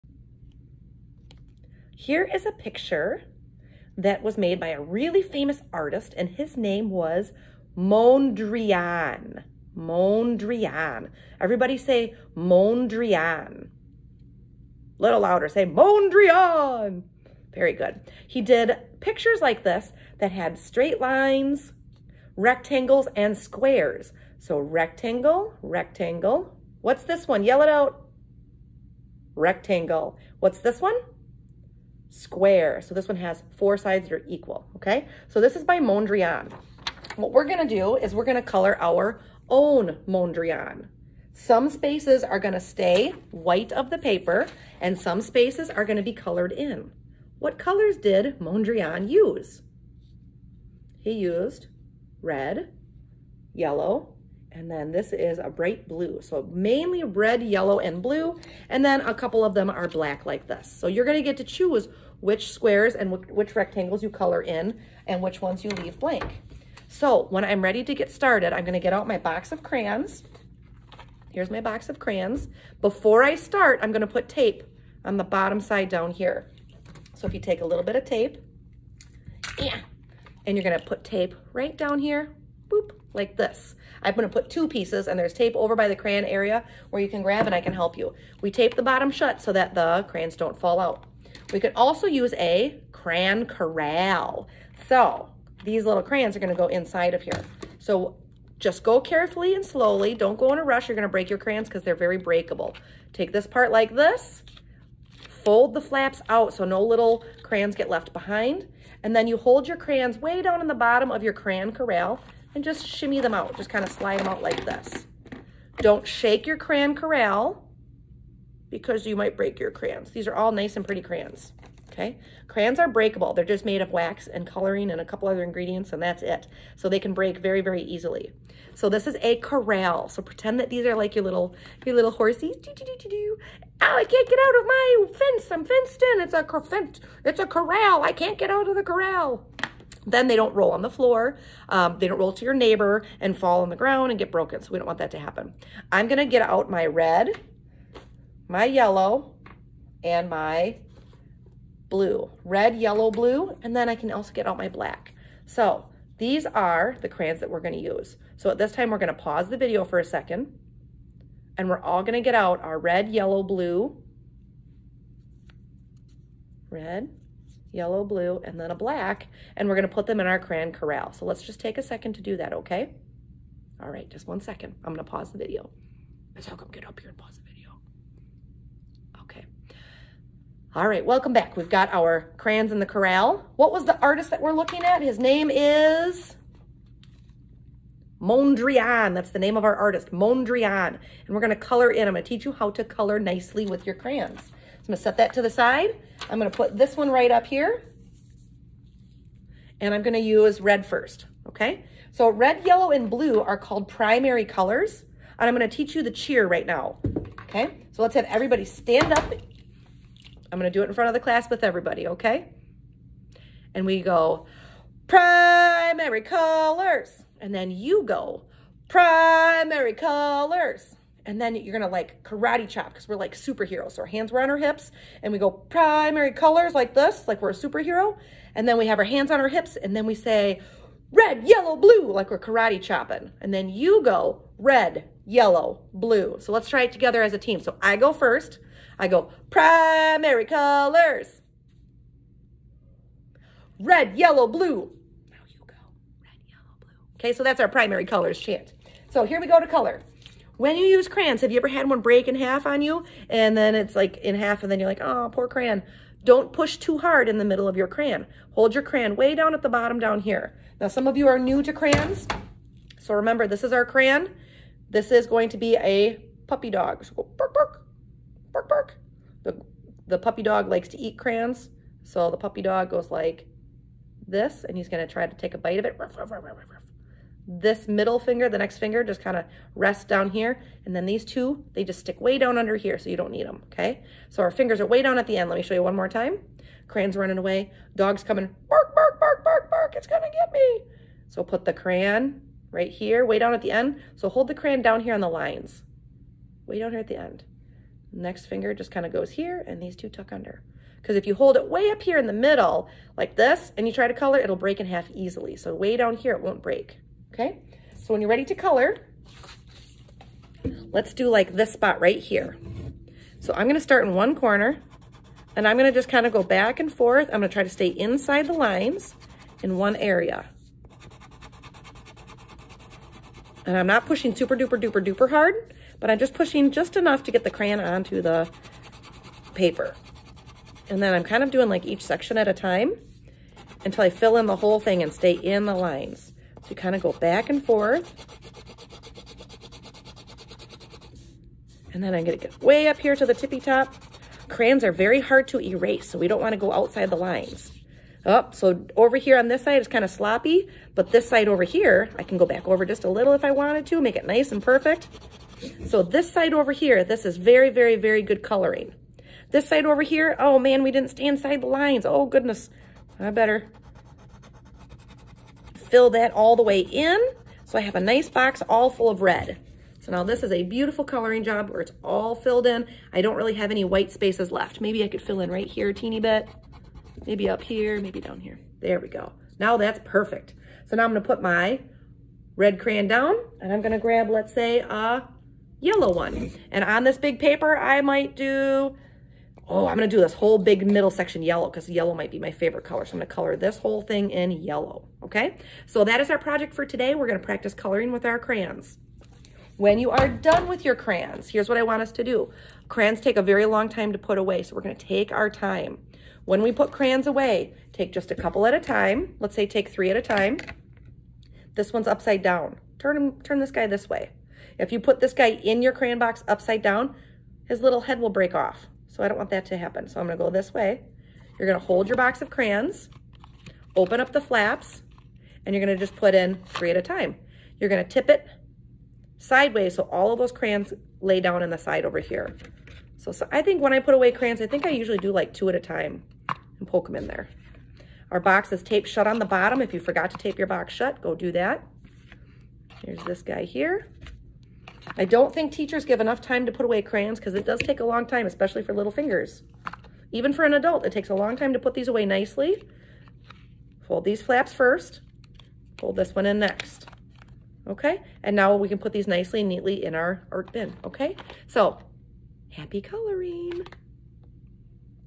How to Color with Crayons - Mondrian
Beginner video on how to color with crayons, use a crayon corral and Create a Mondrian